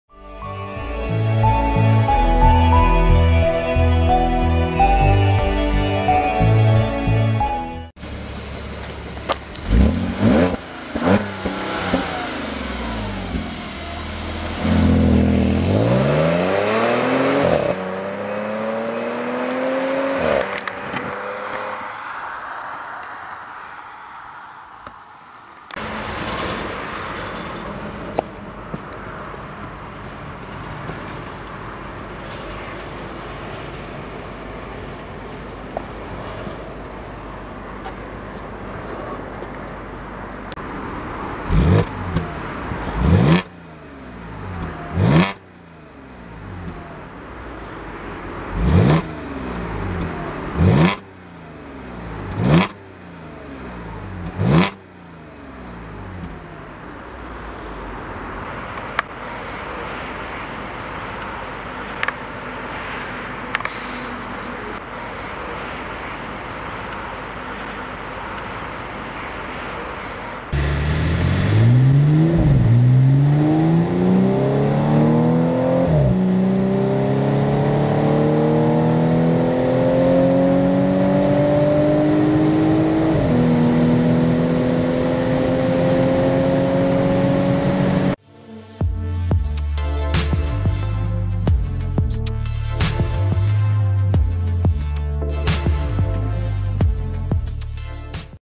Audi S5 Cabrio 3l Turbo 333PS - Sound and 0-160 km/h
Ein Audi Cabrio S5 mit 333 PS sprintet von 0 auf 160 kmh/h.